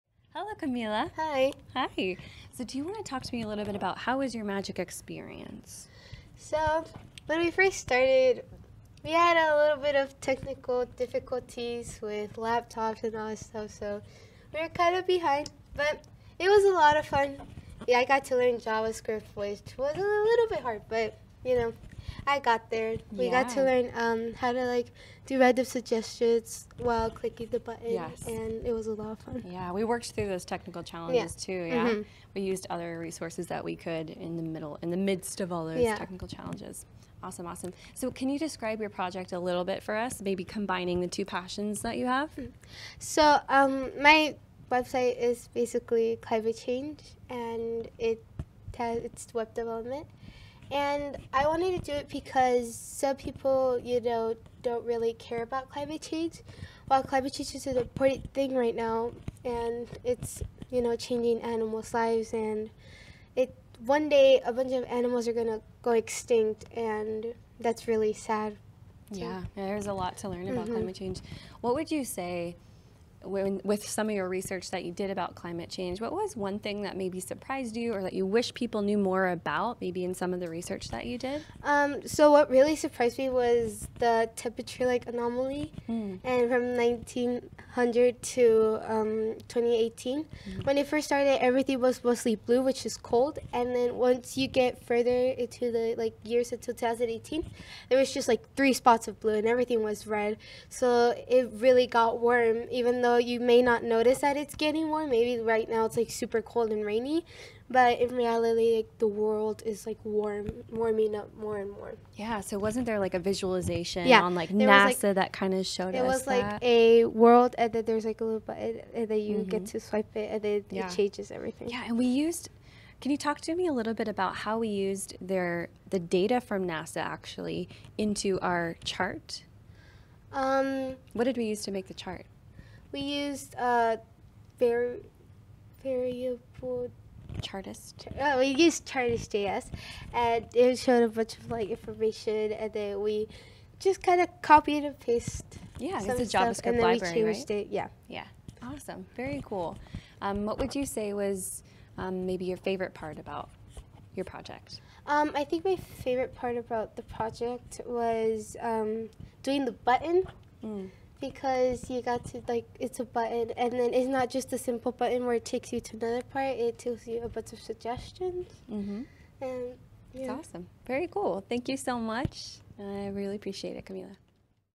Interview with MAGIC mentee Camila C
Cher S interviewing MAGIC mentee Camila C at the 2019 MAGIC Final Presentation event at The Ann Richards School in Austin, Texas in May 2019.

MAGIC is grateful to  Ian Gulliver for producing, shooting, editing, assembling and making available all the videos from this event, and to Cher S for conducting the interviews.